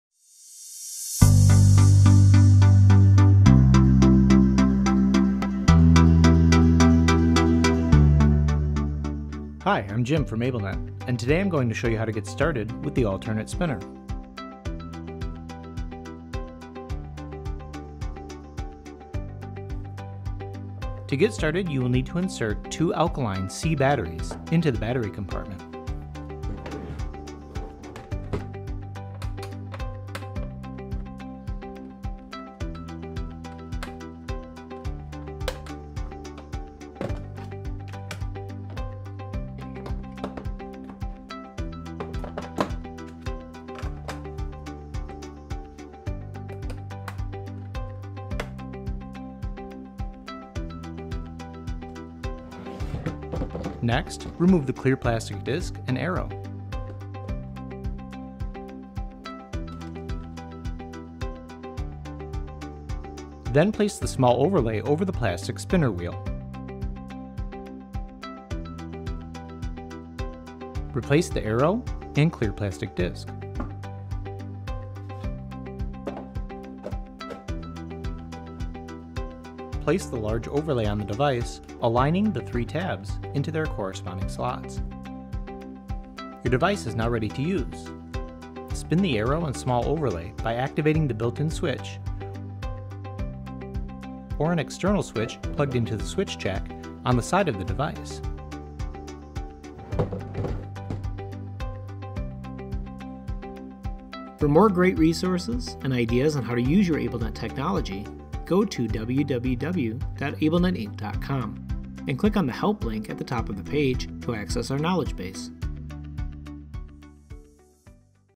Getting Started With The AbleNet All-Turn-It Spinner
Learn how to get started with the AbleNet All-Turn-It Spinner.